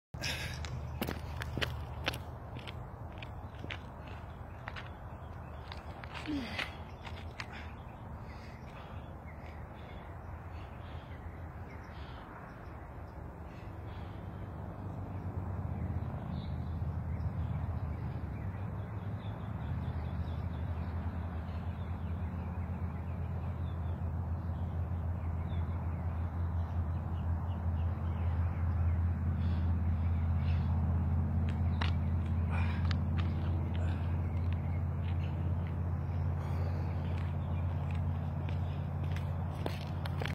Day 5
Day 5 of the 22 push-up challenge to bring awareness to the fact that 22 Veterans die by suicide every day.

I’d like to nominate Carlos Calero Cobo to join in supporting the cause.

Help out by taking the challenge to spread the word. We can help our veterans. Veterans, please seek help. Suicide is not the answer.

1. Once you are nominated your 22 days start the next day.
2. Every day you record yourself doing 22 push-ups. Try your best to reach 22. No judgment.
3. Every day you must nominate a different person. Try to nominate people you think will want to do this and/or have the ability to do it.
4. Enjoy the ride. This is a simple way to bring awareness about a matter that is near to many of us. These brave men and women put their lives on the line to protect those things that we cherish. Many of us know the struggle. You are not alone, you are loved!
I pray for you to find the peace and joy that comes from having hope!